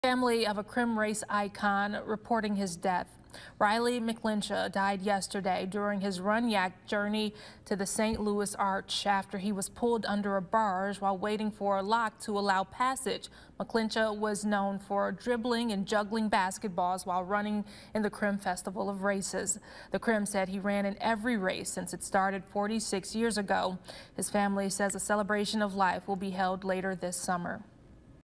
Crim announces death of iconic runner who ‘drubbled’ basketballs during race
A familiar face found running and drubbling - dribbling and juggling - in the Crim Festival of Races died, the Crim Fitness Foundation said.